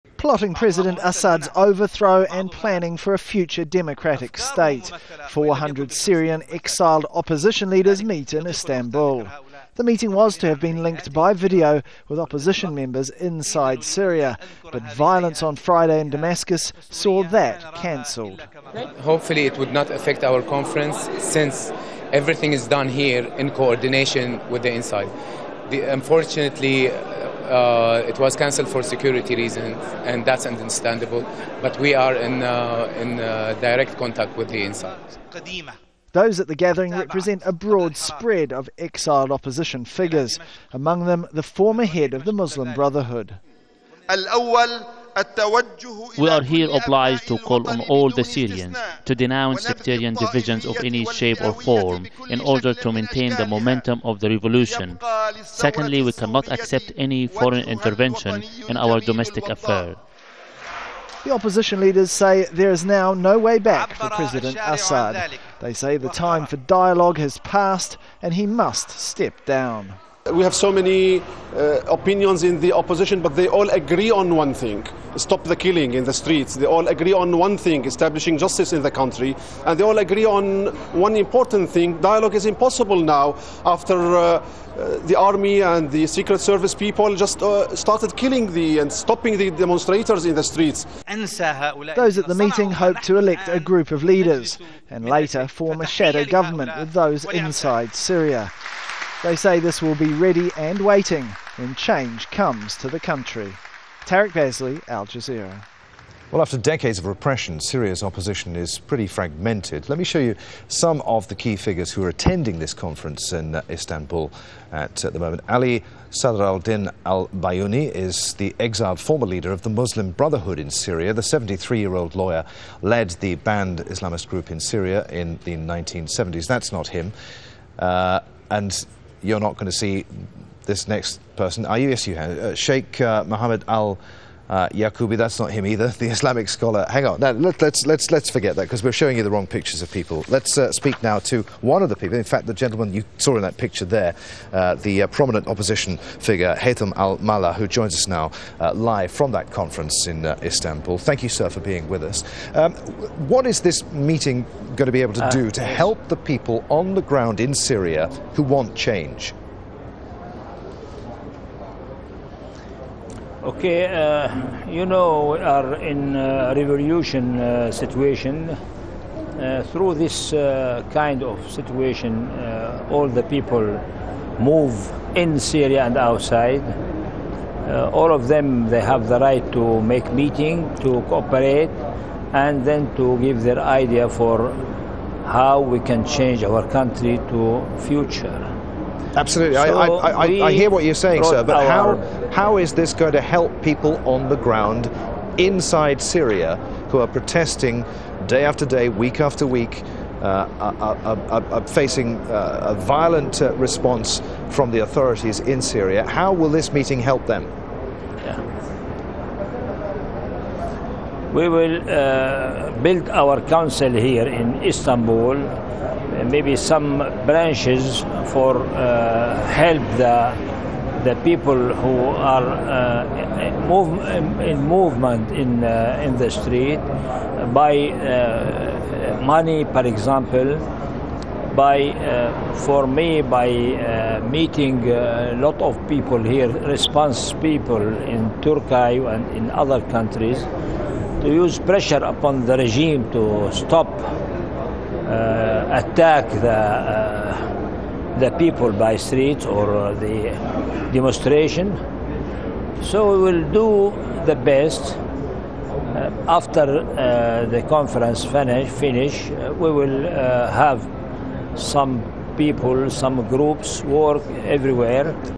Syrian opposition leaders meet in Turkey
At least 400 members of Syrian opposition, divided between conservatives Islamists and liberals, are attending the so called National Salvation Congress in Istanbul on Saturday to try to unite behind the goal of ending 41 years of Assad family rule.
Al Jazeera's Tarek Bazley reports.